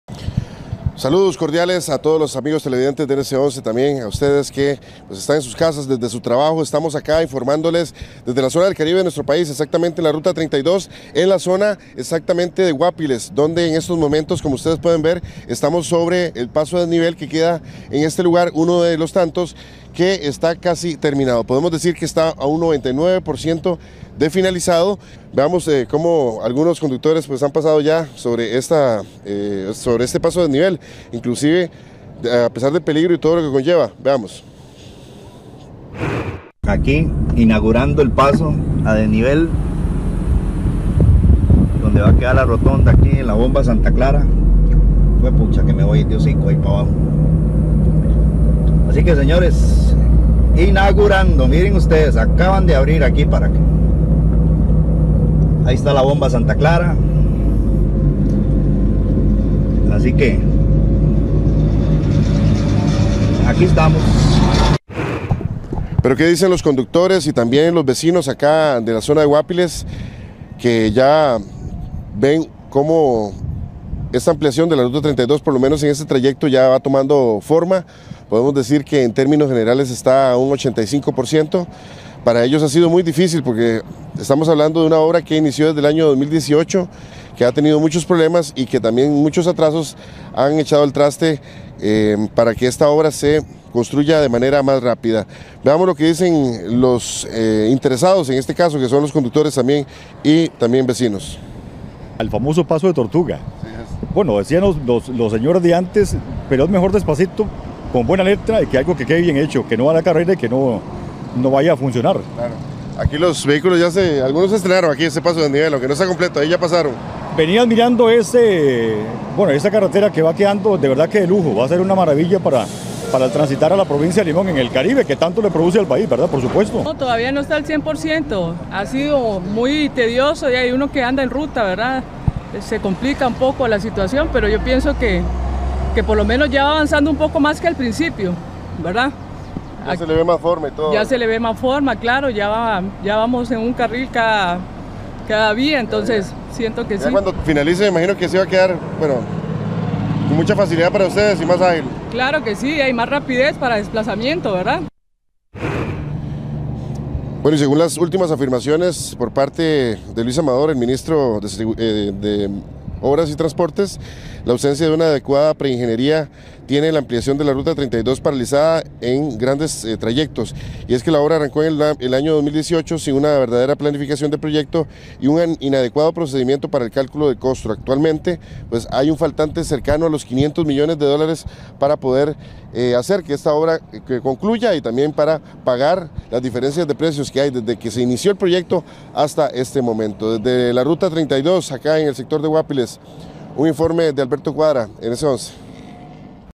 Conductores estrenaron paso en Ruta 32 a pesar de no estar terminada
Autoridades recomiendan a los conductores tener mucha precaución.
Muchos conductores registraron su primera impresión al momento de conducir por dicho sector. Por otro lado, el jerarca del MOPT, Luis Amador, indicó que la ausencia de una adecuada pre ingeniería es la causante de las demoras en la Ruta 32.